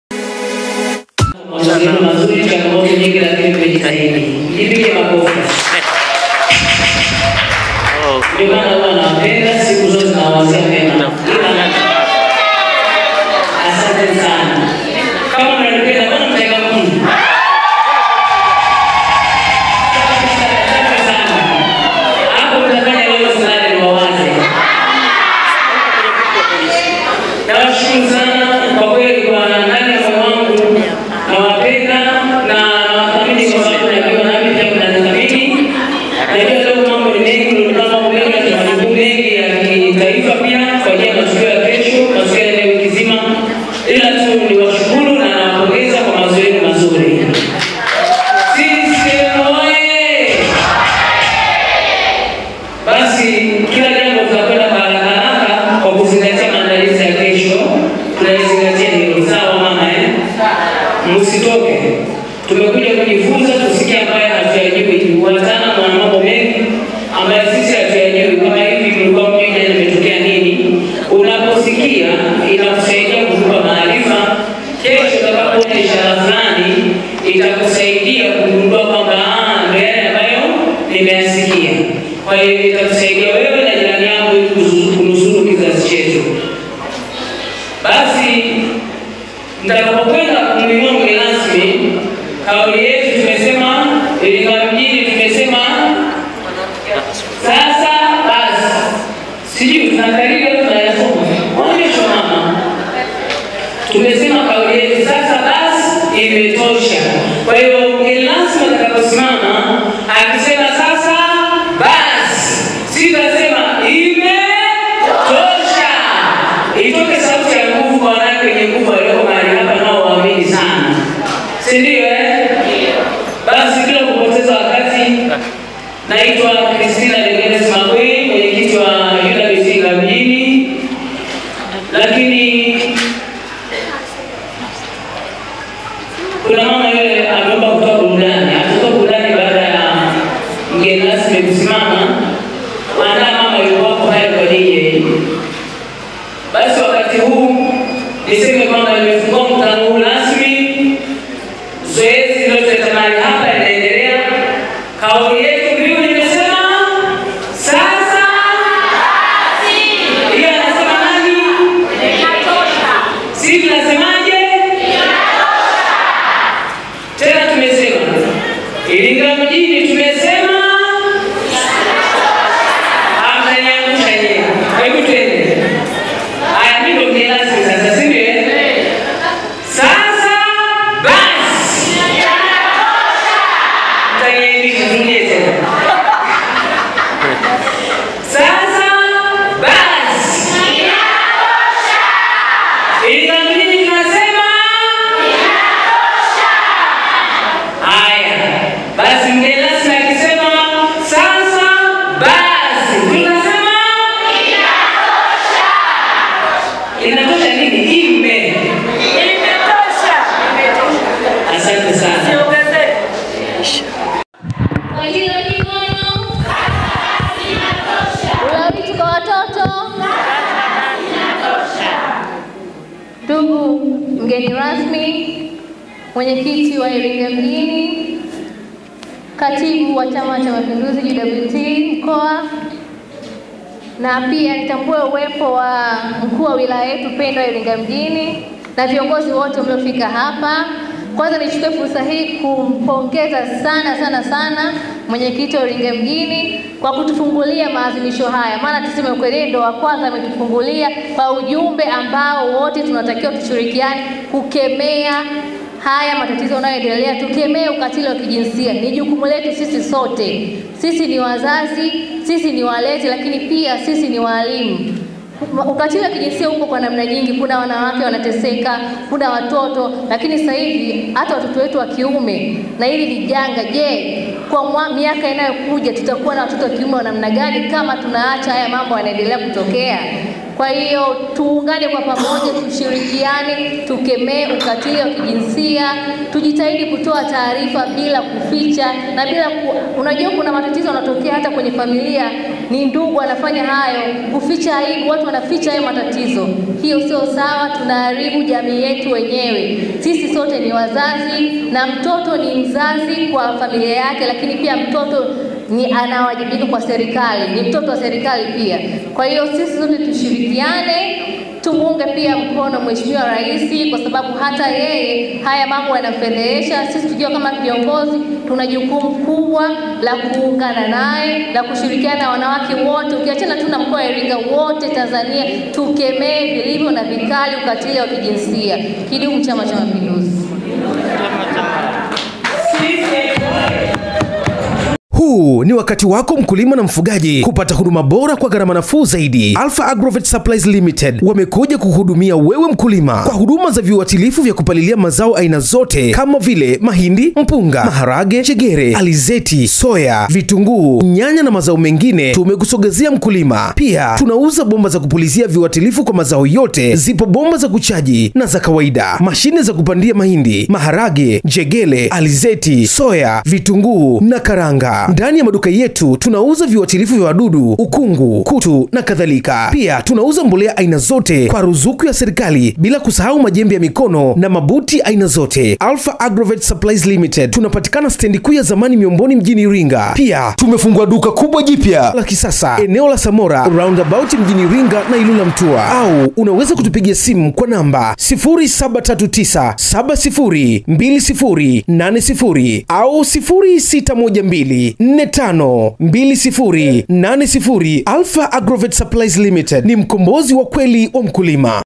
MWENYEKITI UWT IRINGA MJINI CHRISTINA MAKWEYI NA FATMA REMBO WATOA UJUMBE HUU ...
Mwenyekiti wa UWT wilaya ya Iringa mjini Christina Makweyi

Usisahau kusubscribe